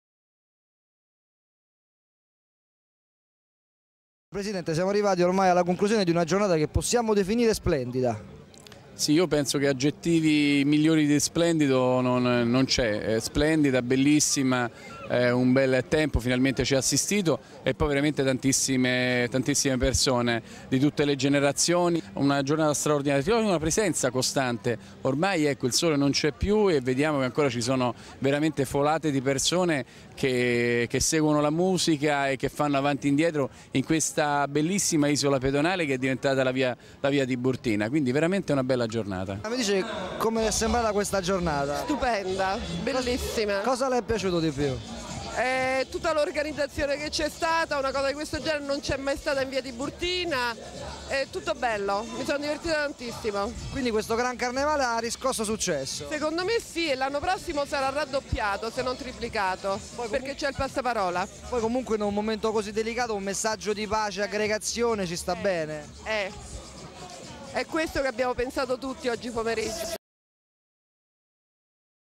Gran Carnevale Romano - Interviste Commenti e...
Intervento del Presidente IVANO CARADONNA al "Gran CARNEVALE ROMANO" - 2003. Edizione dedicata alla PACE.